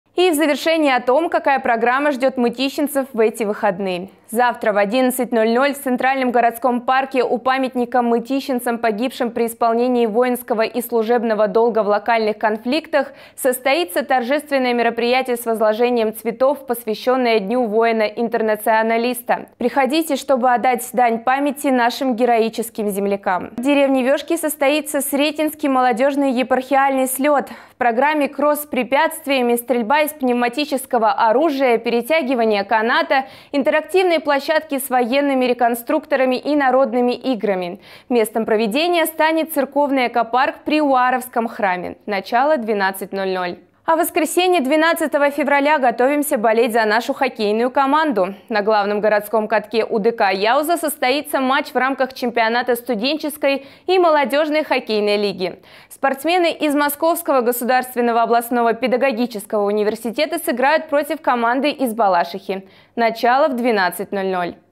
Какая программа ждет мытищинцев на выходных?
Завтра в 11.00 в Центральном городском парке у памятника «Мытищинцам, погибшим при исполнении воинского и служебного долга в локальных конфликтах» состоится торжественное мероприятие с возложением цветов, посвященное Дню воина-интернационалиста.
Подробнее о мероприятиях – в нашем ролике. (12+)